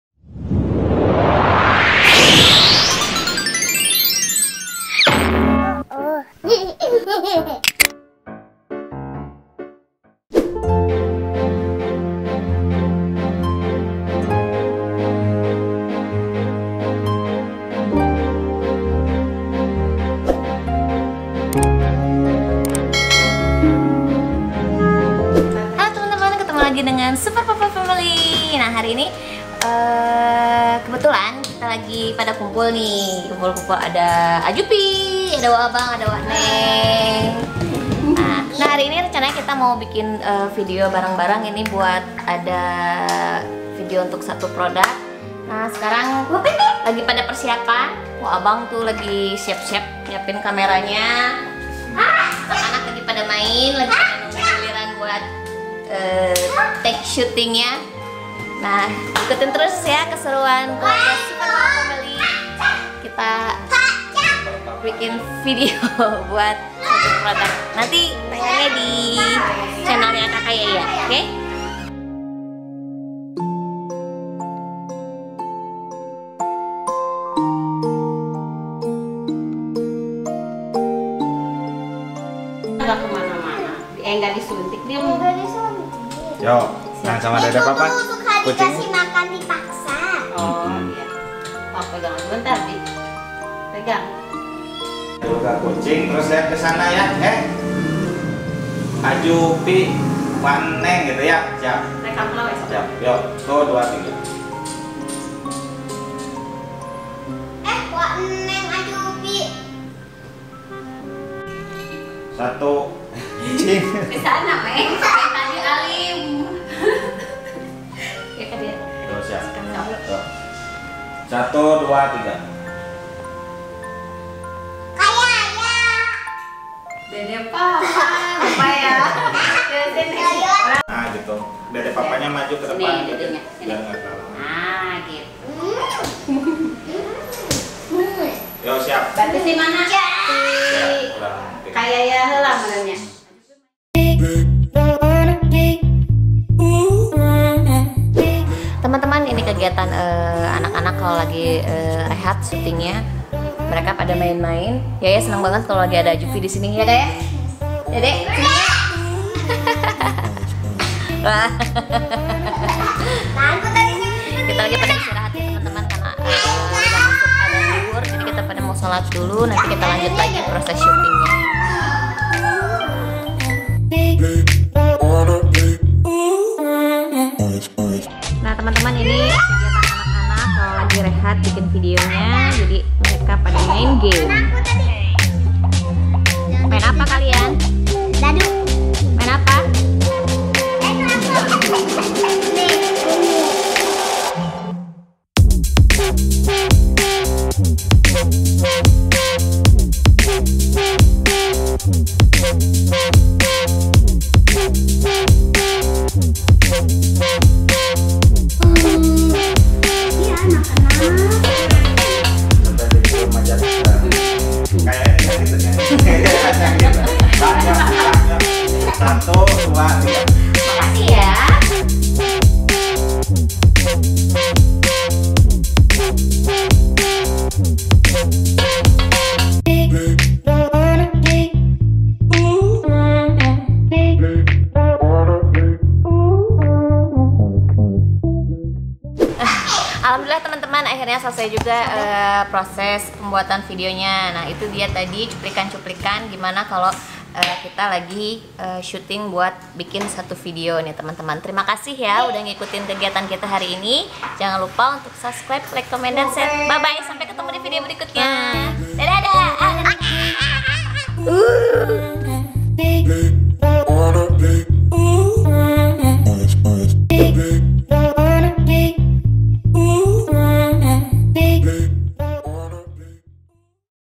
Behind The Scene Surprise Hadiah Spesial di Hari Ibu !!
Hai gaes... Kali ini keluarga super fafa dan keluarga azufi lagi bikin video bareng nih... Kebetulan lagi ada project endorse video buat salah satu produk sejadah An-Najah by Custom Kids... Nah berikut ini keseruan-keseruan saat pembuatan video drama inspiratif suprise hadiah spesial di hari ibu... Mau tau akting kak yaya, azufi dan dede fafa saat lagi bikin video? yuk tonton videonya sampai selesai...